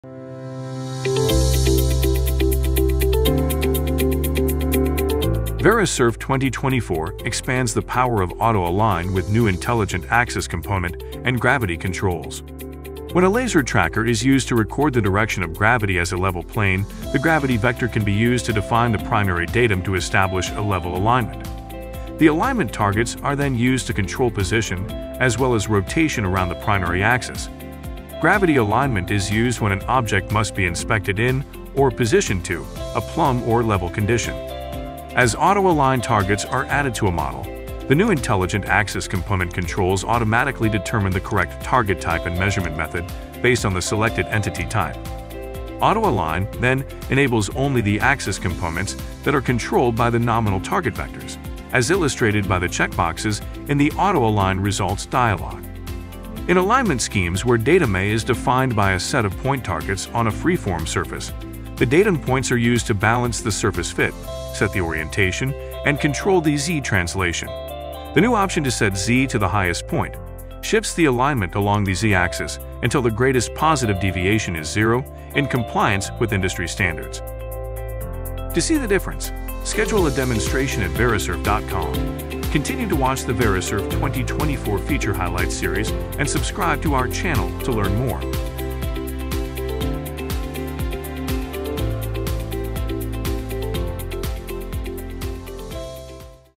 Verisurf 2024 - Feature Highlights | New Auto Align Controls
✅ Hold Gravity Level for Laser Tracker applications.
✅ Intelligent axis component controls to achieve desired alignment strategy.
✅ Set highest point, to constrain the alignment to the highest Z measurement when aligning to a freeform surface as a primary datum.

👍Click the like button on all new videos.

Verisurf 2024 expands the power of auto-a-line with new intelligent axis component and gravity controls. When a laser tracker is used to record the direction of gravity as a level plane, the gravity vector can be used to define the primary datum to establish a level alignment. The alignment targets are [then] used to control position as well as rotation around the primary axis. Gravity alignment is used when an object must be inspected in, or positioned to, a plumb or level condition. As auto-a-line targets are added to a model, the new intelligent axis component controls automatically determine the correct target type and measurement method based on the selected entity-type. Auto-a-line then enables only the axis components that are controlled by the nominal target vectors, as illustrated by the check boxes in the auto-a-line results dialog. In alignment schemes where datum-A is defined by a set of point-targets on a freeform surface, the datum points are used to balance the surface fit; set the orientation; and control the Z-translation. The new option to Set 'Z' to the highest point, shifts the alignment along the Z-axis until the greatest positive deviation is zero, in compliance with industry standards.